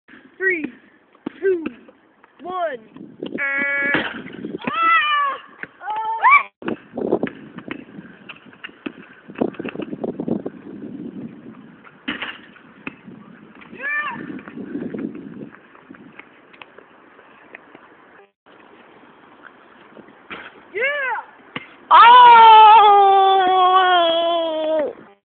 Cool long shots (takin on my phone so its bad quality)
Me and my friend takin long shots(recorded on phone)